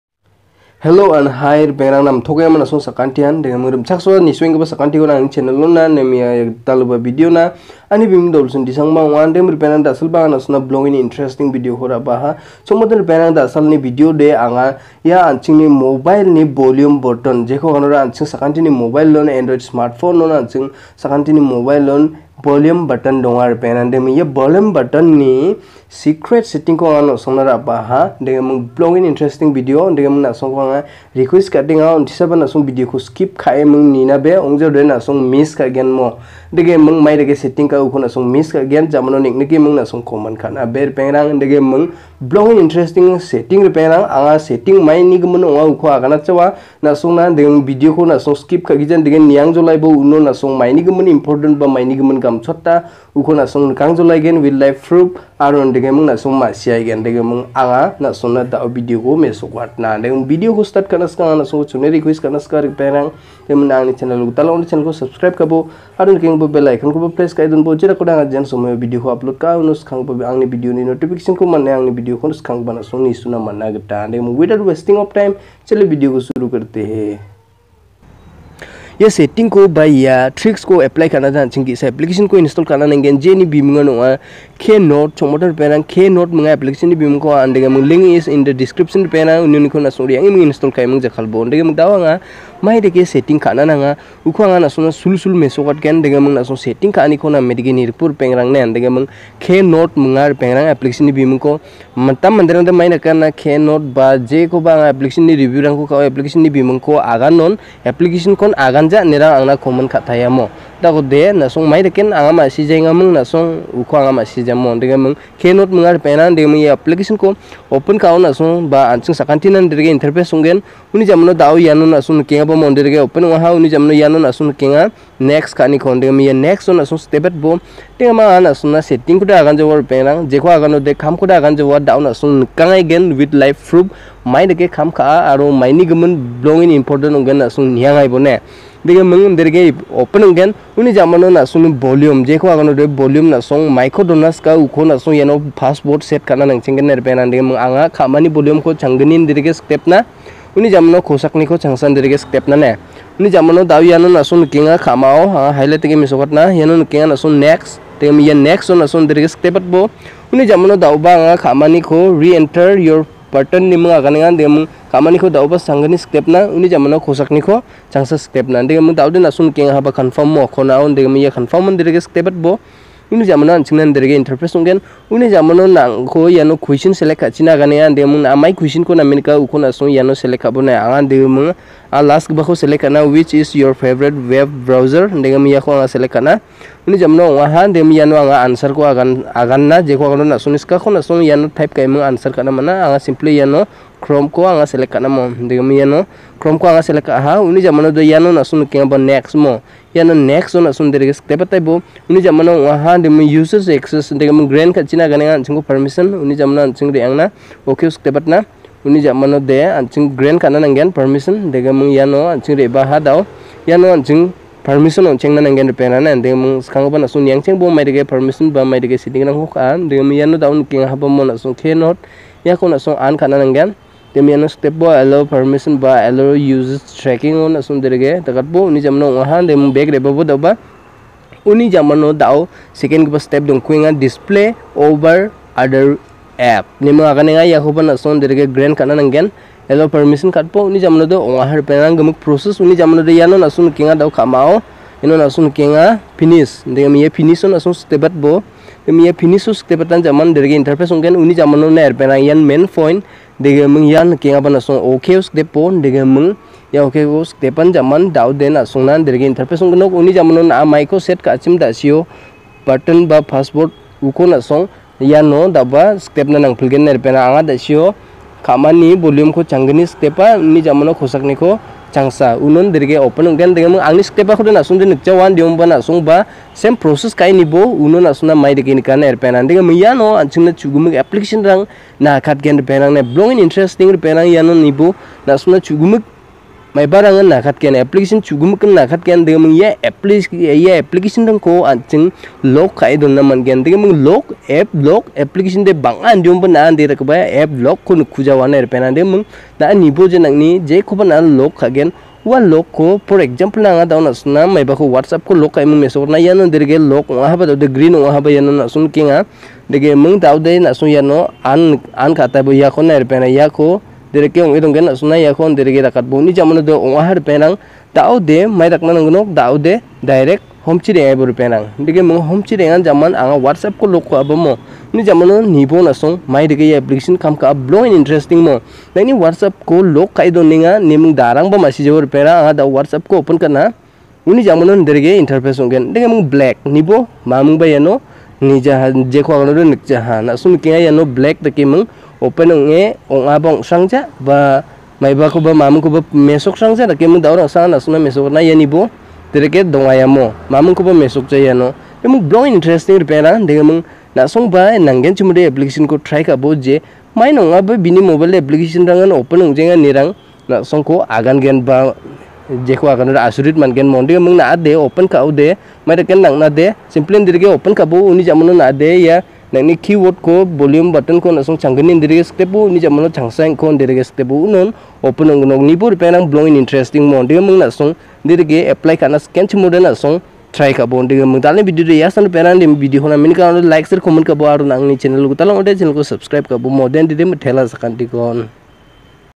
Volume button secret trick //Volume button ni gasugipa tricks
Rimchaksoa sakantikon Ia video ona indakeming da.alni video de maikai anching volume button ko secret setting ka.ai jakalna nanga iani gimin video onga iako jakalnade ge1 application nanggen k note app onga bilongen useful app nangode jakalbo mo

     Mitela

About channel. In this channel you can see watch any category of videos.

Follow me//